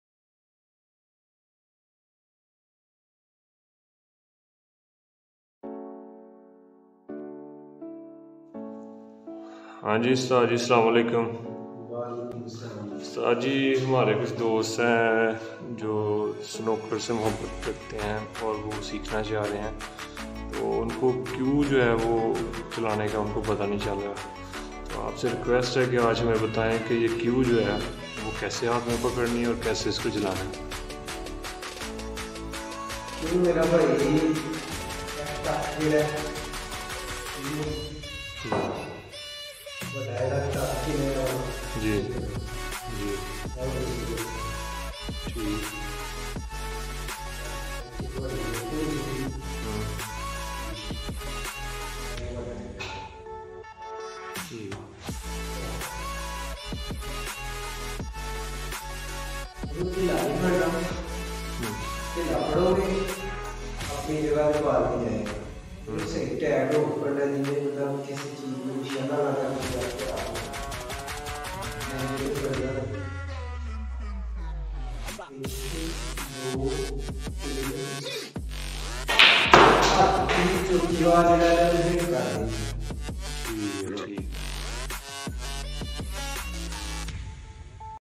very informative tips given by ustad jamshid on cue(stick) 🔥
a breif discussion on how to use a cue while playing snooker. concentration matters🔥🔥